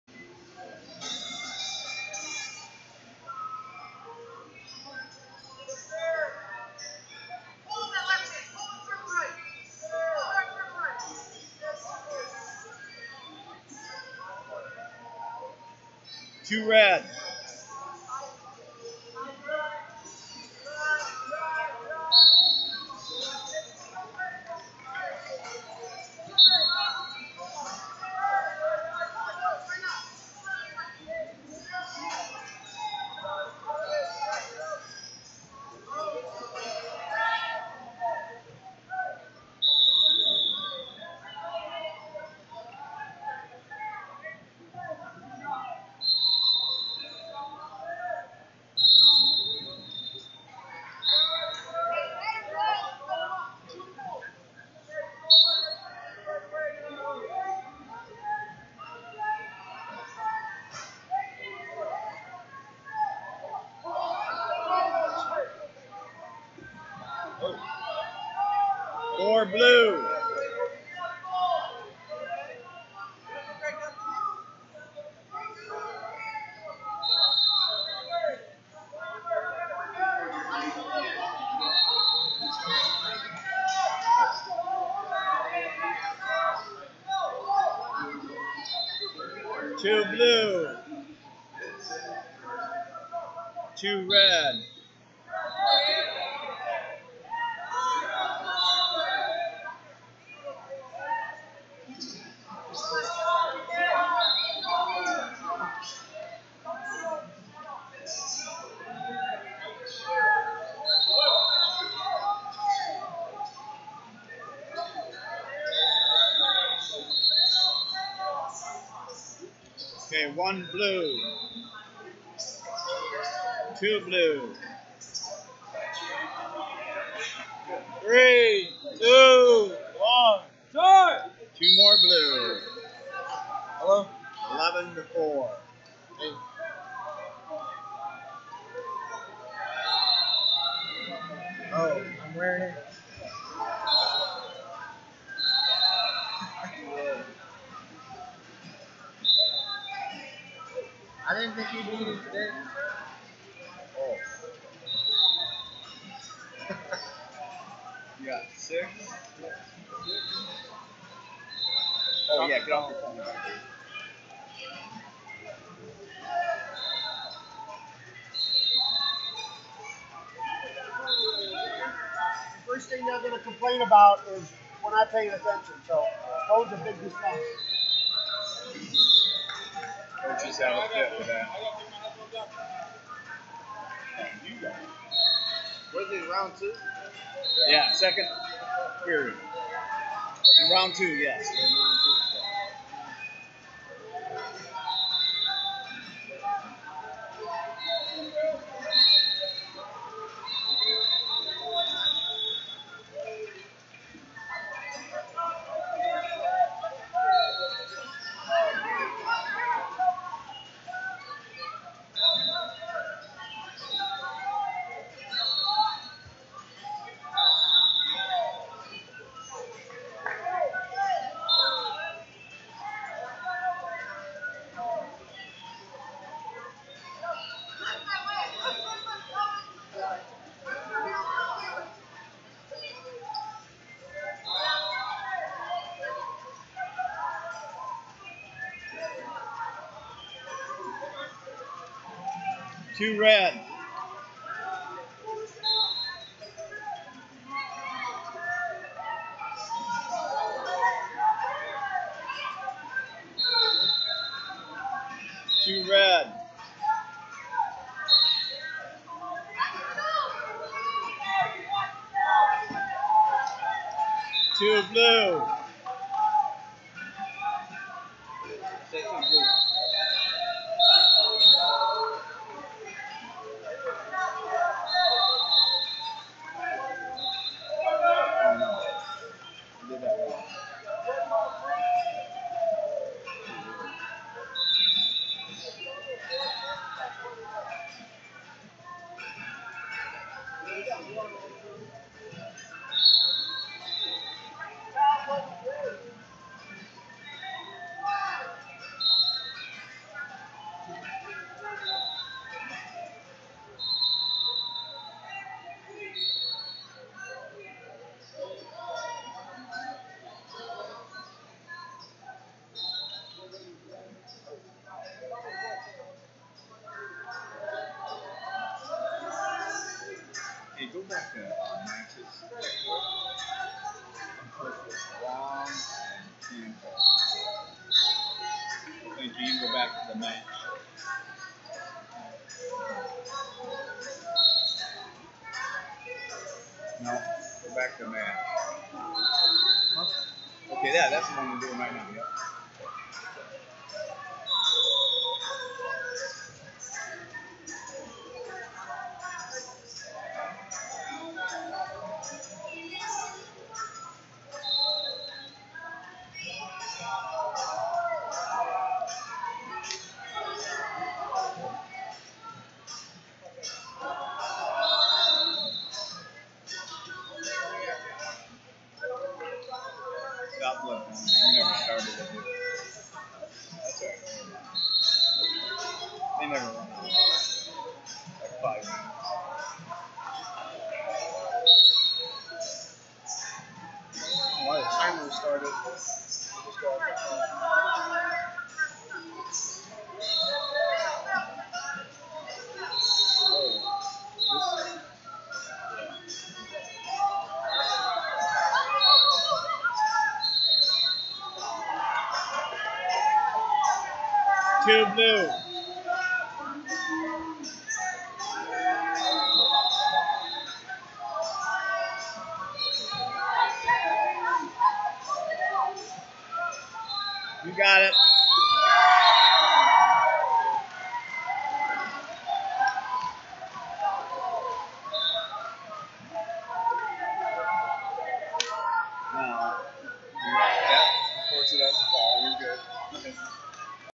Mat 9 130 Travis Smith Minnesota Red Vs Austin Richens Utah
2019 14U Schoolboy Duals - Mat 9 130 Travis Smith Minnesota Red Vs Austin Richens Utah